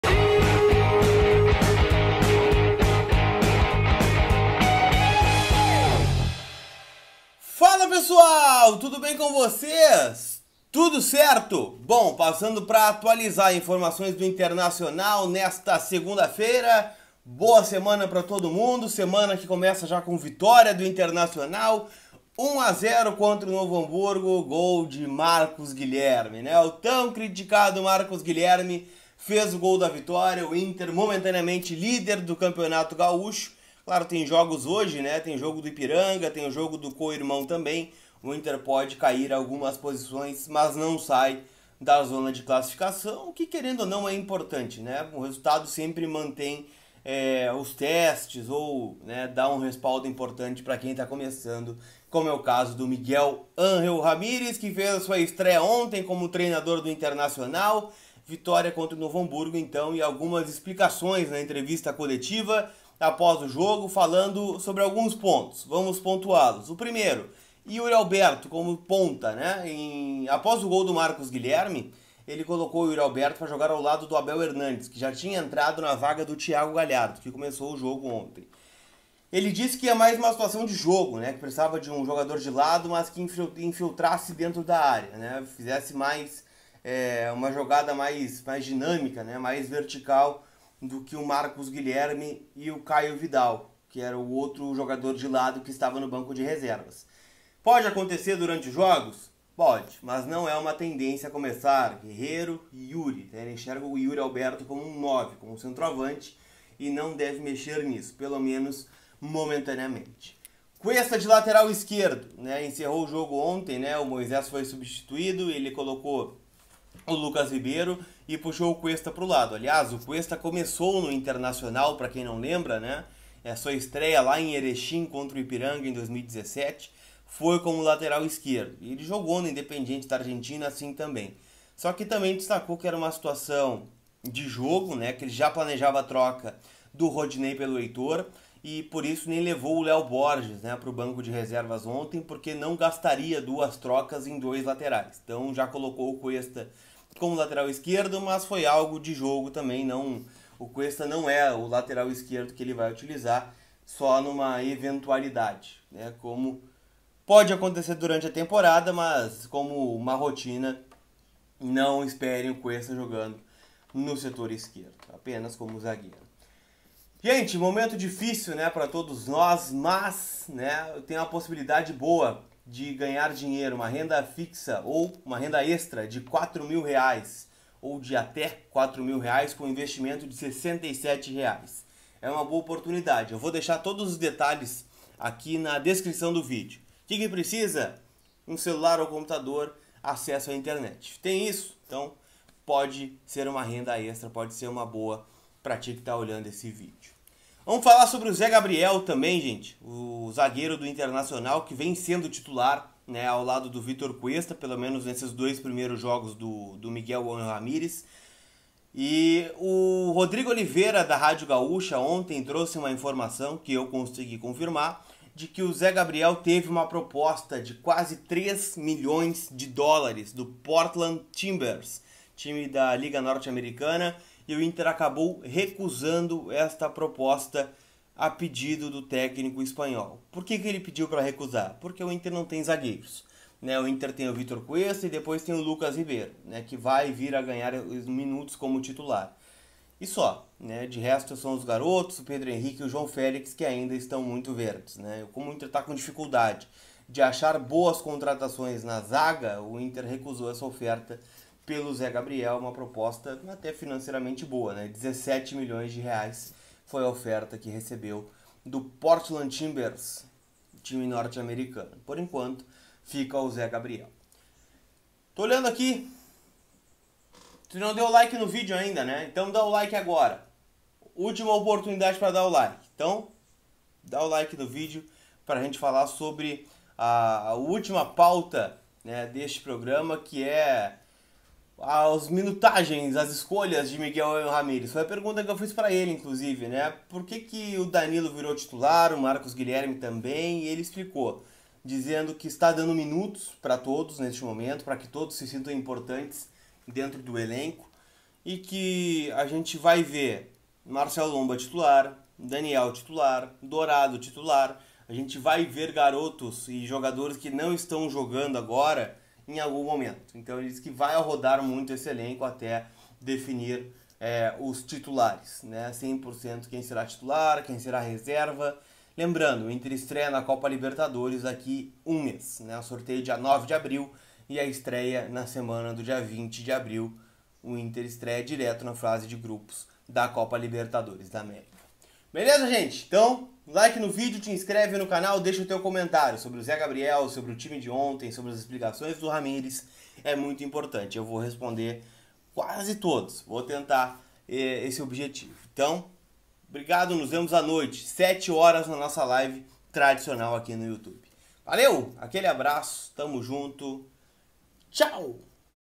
INTER RECUSA OFERTA POR ZÉ GABRIEL | CUESTA LATERAL E YURI PONTA: RAMIREZ EXPLICA | TODOS VÃO JOGAR
VITÓRIA COLORADA: Inter bate o Novo Hamburgo por 1 a 0 com gol de Marcos Guilherme. Após o jogo, o estreante Miguel Ángel Ramirez concedeu entrevista e explicou algumas questões importantes sobre tática e escolhas de equipe.